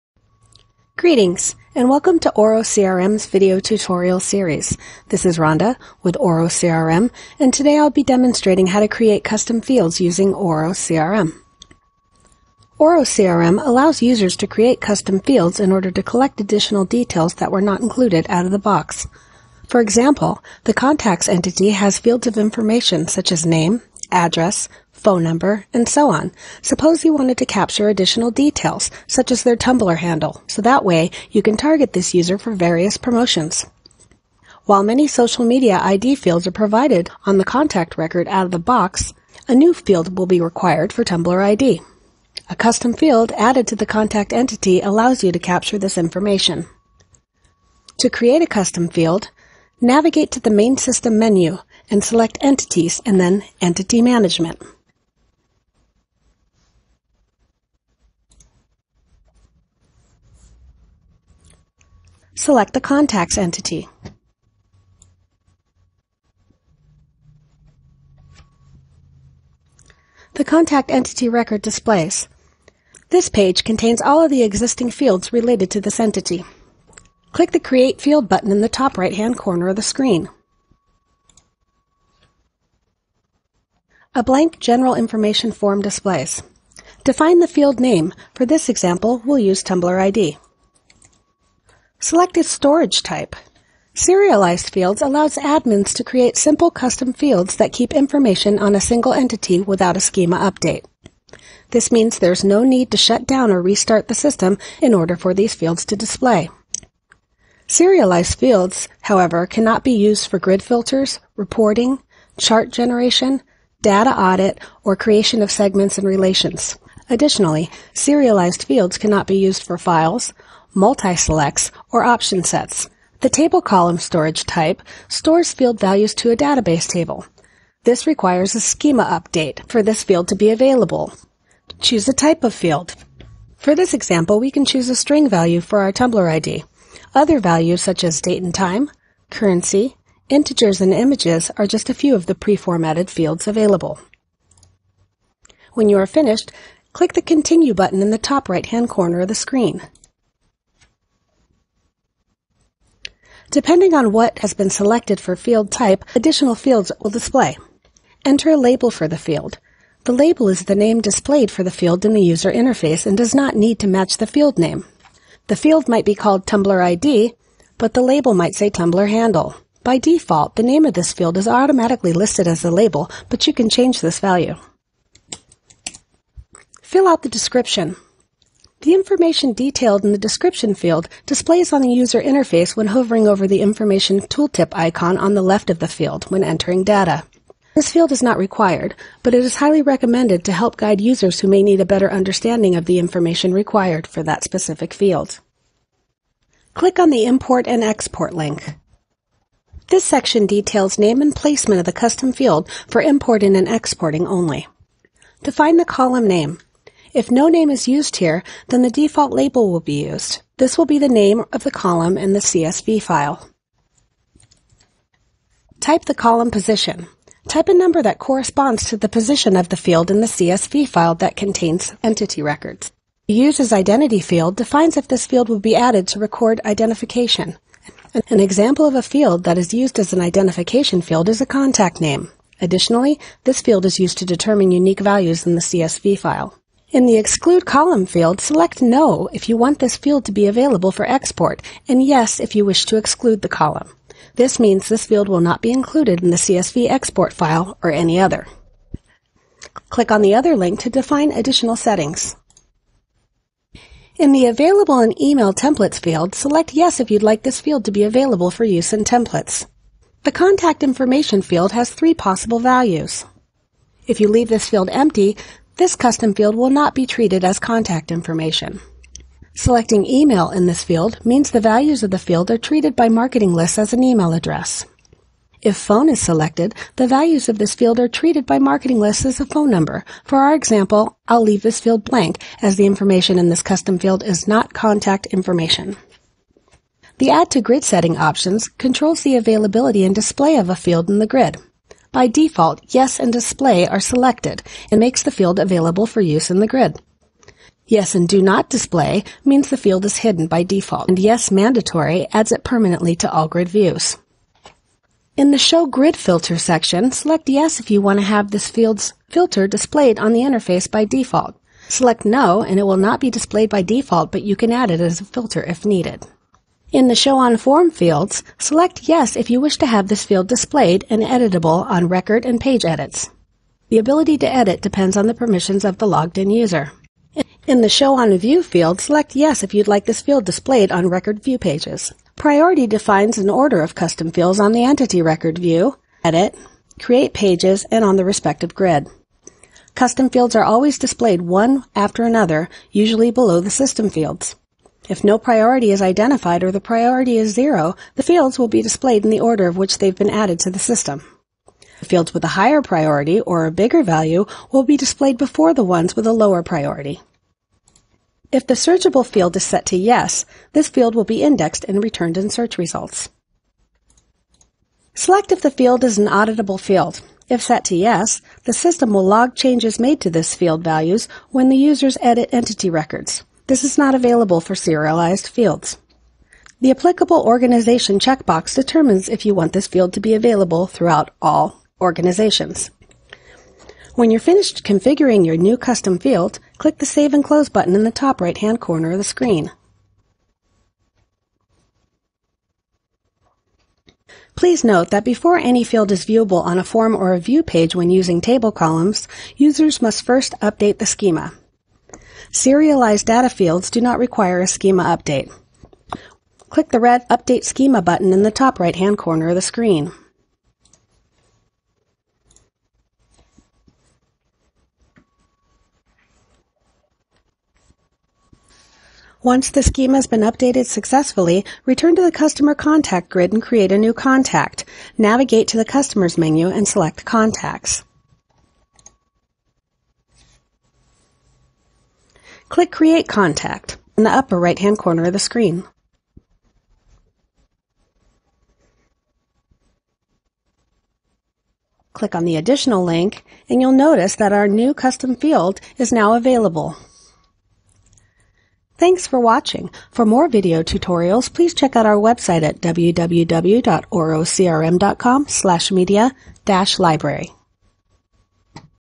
How to Create A Custom Field in OroCRM
In OroCRM, you can create custom fields for an entity to populate them with extra info. For example, you may need to capture additional customer data to use for, say, promotions. Watch this video to learn how to configure a new custom field in the application.